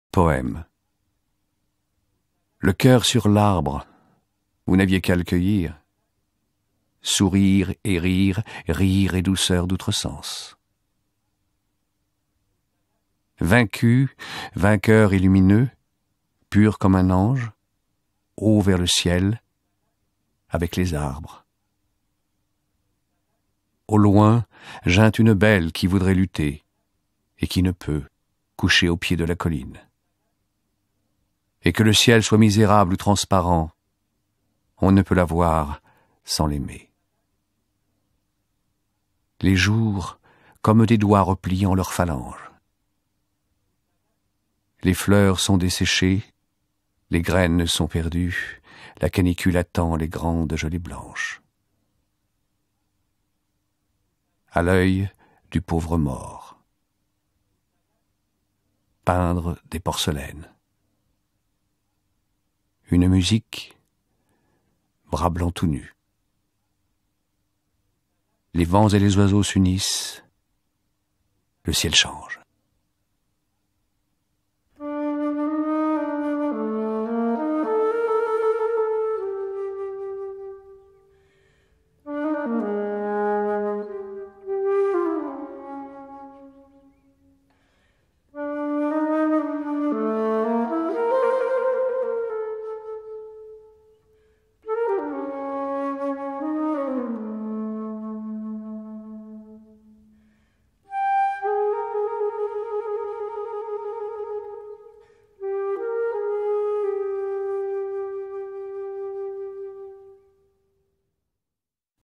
Poèmes, Paul Éluard
lu par Gérard Desarthe
in Capitale de la Douleur